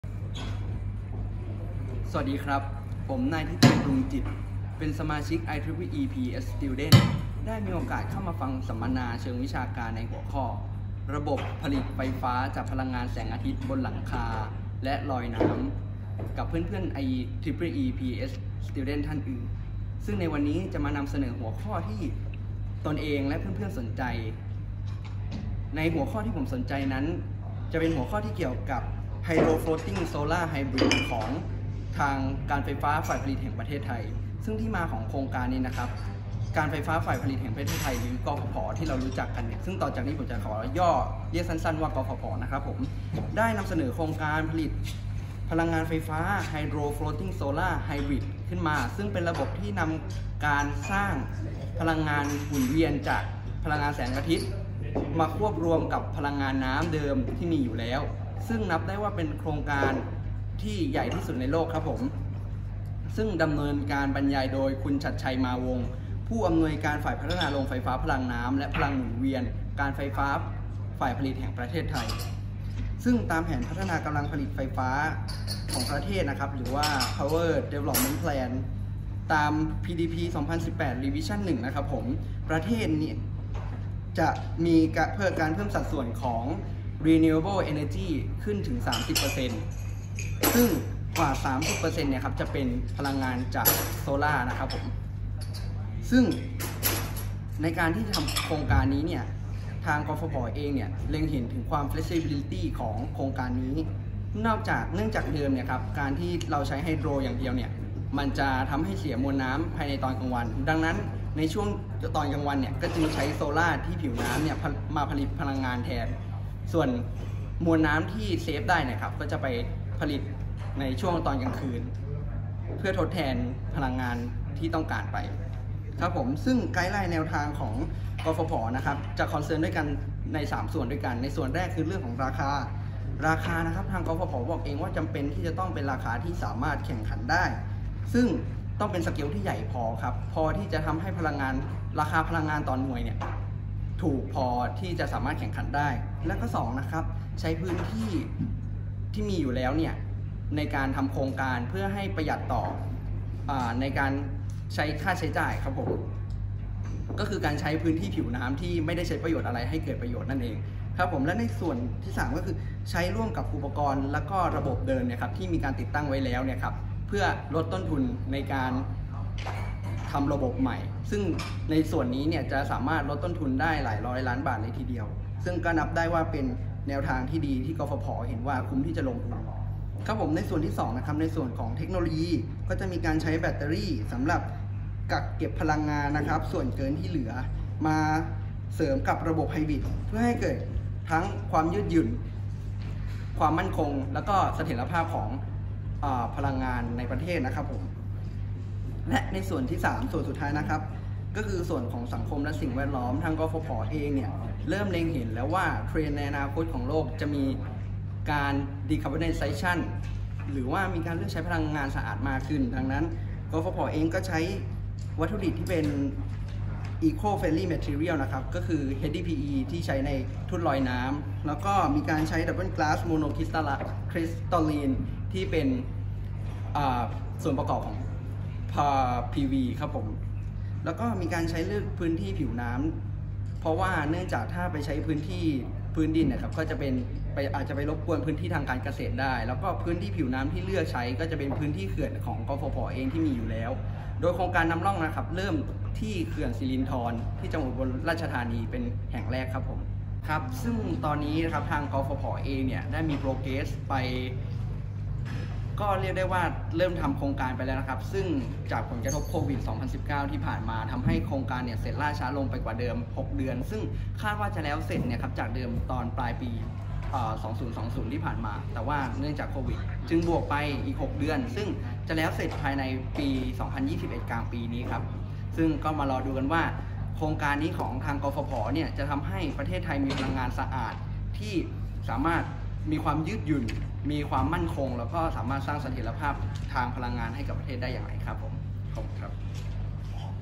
KMUTT Apprentice @ กรณีศึกษา โครงการโซลาร์ฟาร์มลอยน้ำของการไฟฟ้าฝ่ายผลิตแห่งประเทศไทย
สรุปการสัมนา IEEE Power & Energy Series: Renewable Energy
เรื่องระบบผลิตไฟฟ้าจากพลังงานแสงอาทิตย์บนหลังคา (Solar Rooftop) และลอยน้ำ (Floating)

ในหัวข้อ กรณีศึกษา โครงการโซลาร์ฟาร์มลอยน้ำของการไฟฟ้าฝ่ายผลิตแห่งประเทศไทย
บรรยายโดย คุณฉัตรชัย มาวงศ์ ผู้อำนวยการฝ่ายพัฒนาโรงไฟฟ้าพลังน้ำและพลังงานหมุนเวียน การไฟฟ้าฝ่ายผลิตแห่งประเทศไทย

สรุปโดย นายธิติ ปรุงจิตต์
มหาวิทยาลัยเทคโนโลยีพระจอมเกล้าธนบุรี
Mr. Thiti Prungjit
King Mongkut's University of Technology Thonburi
IEEE PES Membership Student No. 97541753